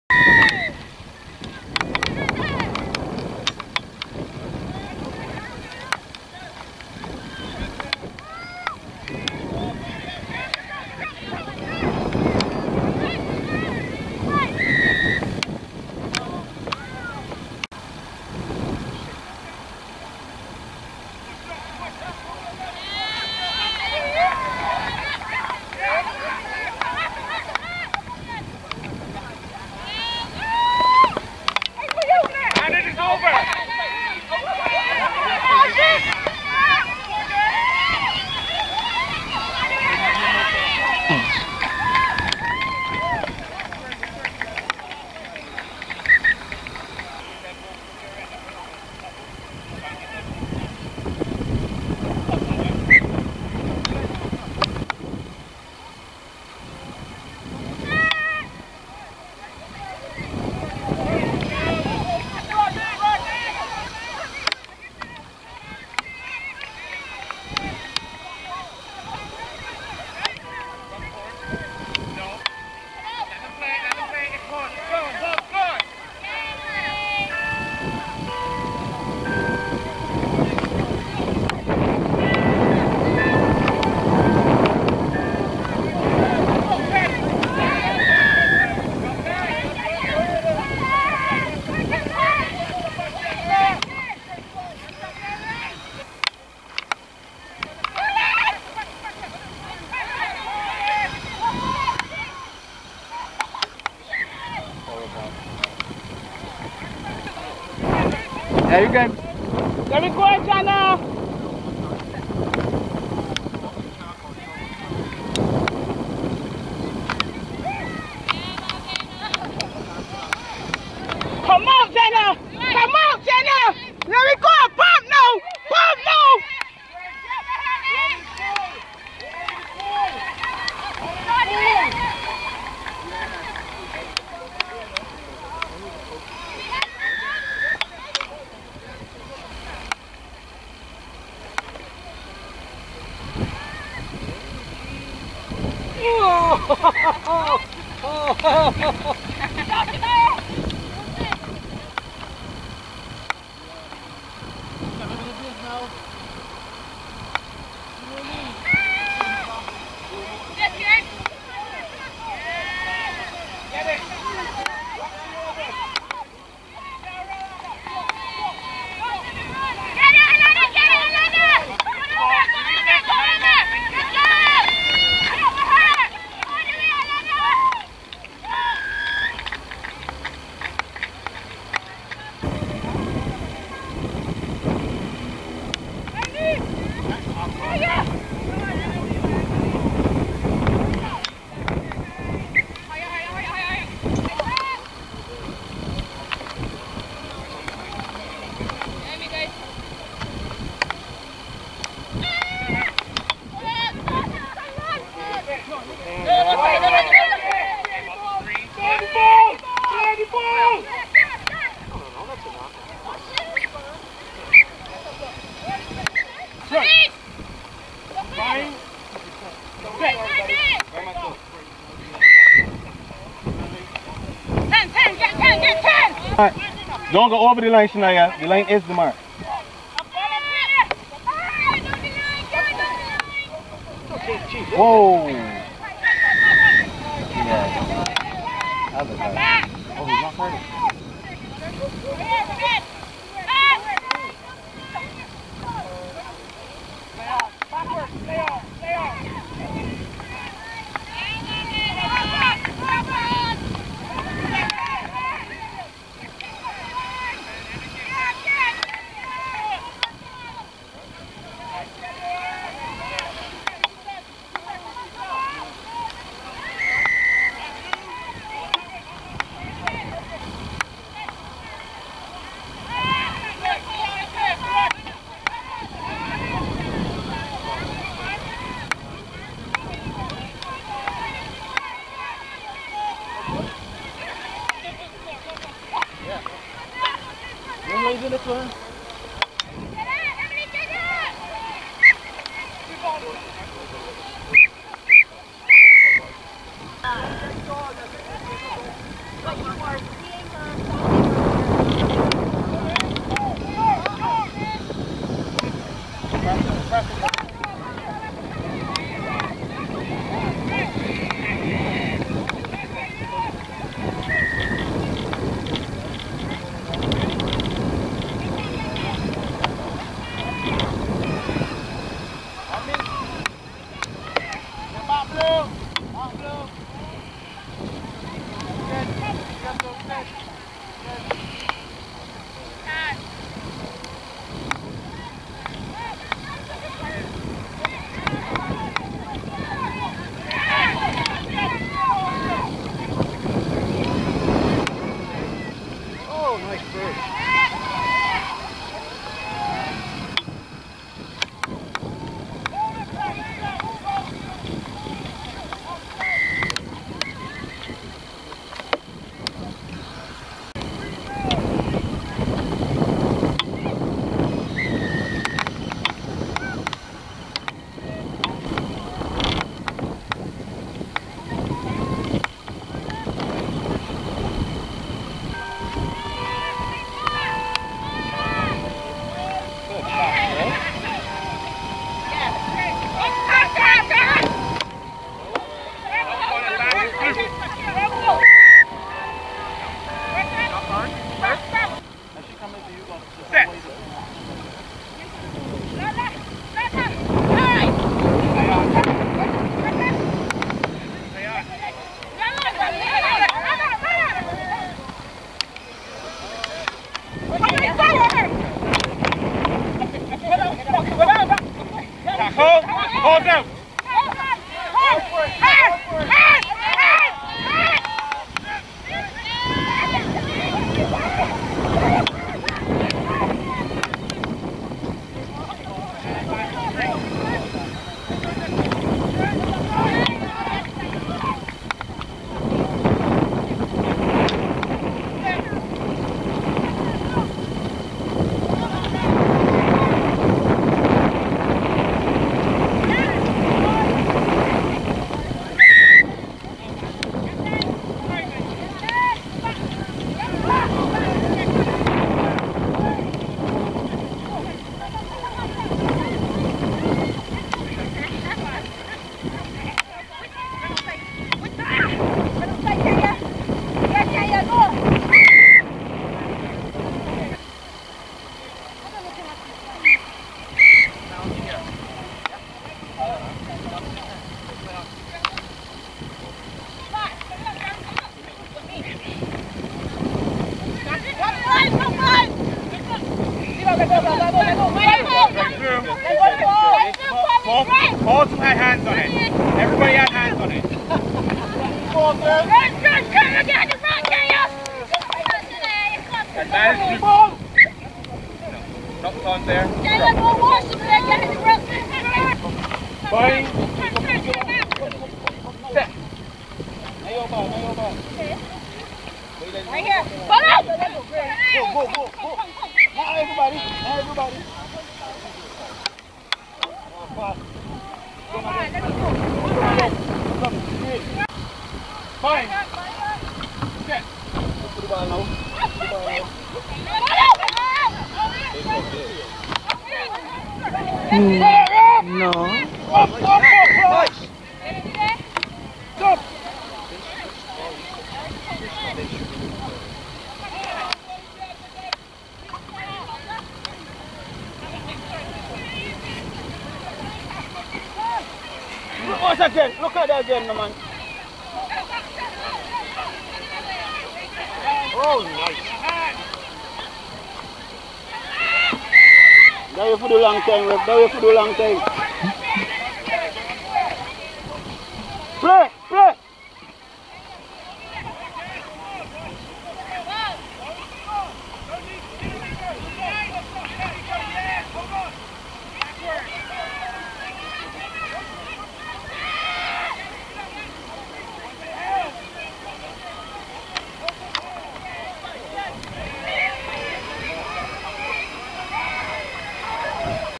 Bajan Pride vs St Vincent Women
Island 7s Rugby Tournament hosted by H1X1 Barbarians Rugby, Barbados.
Ladies Match 1: Bajan Pride 24, St Vincent Women 0